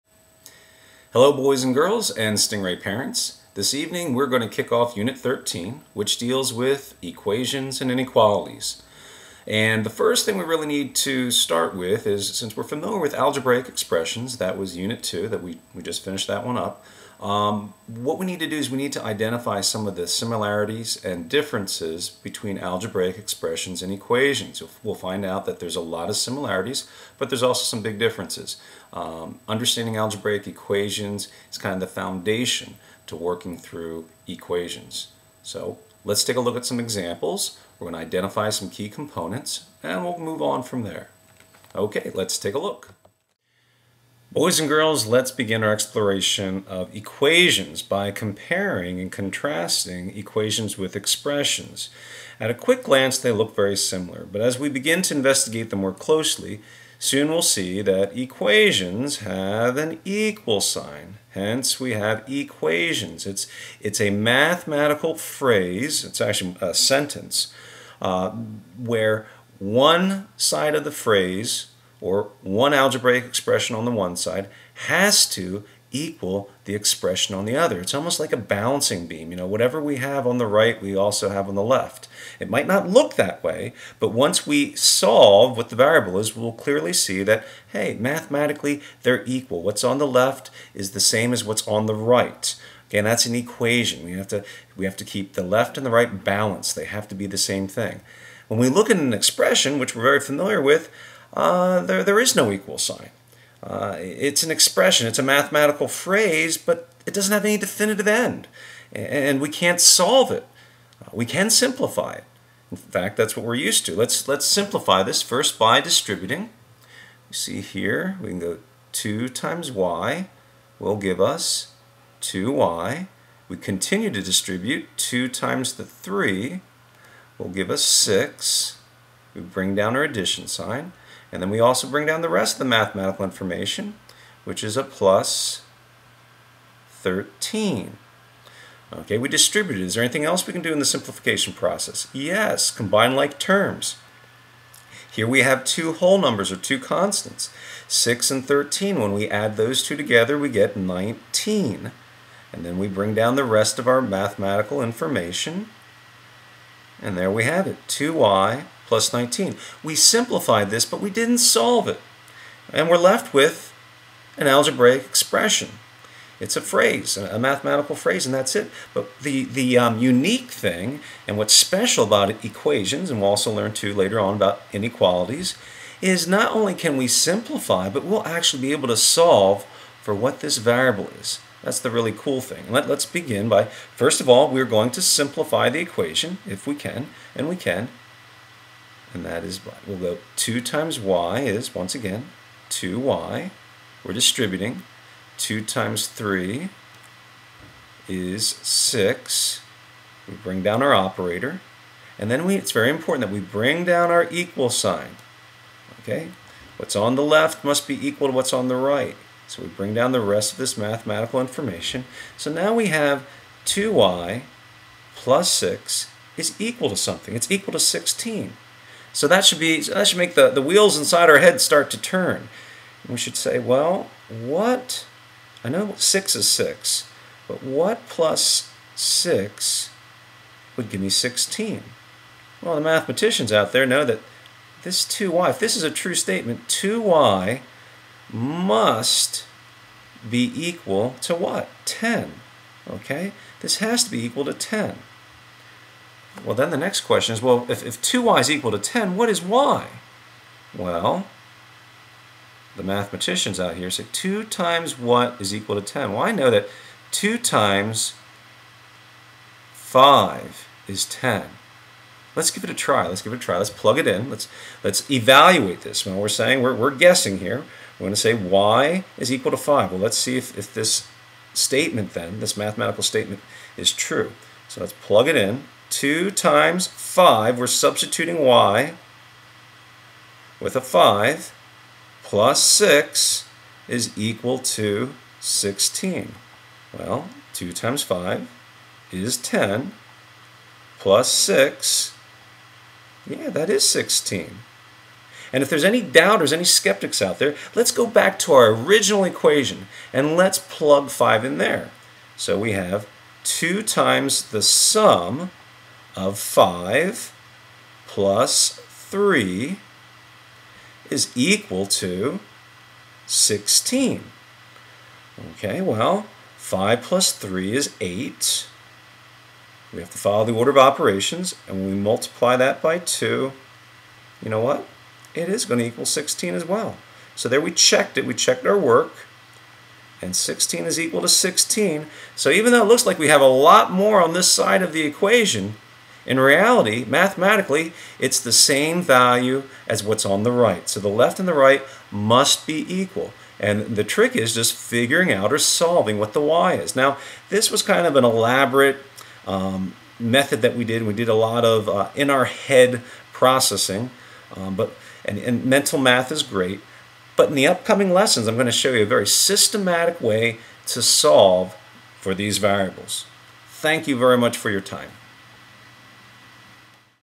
Equations vs Expression
This lesson introduces the similarities and differences between equations and expressions.